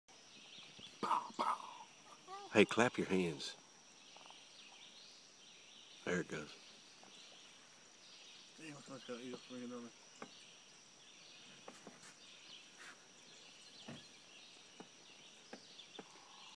Green River Kentucky bald eagle
Bald eagle at Green River in Kentucky 2013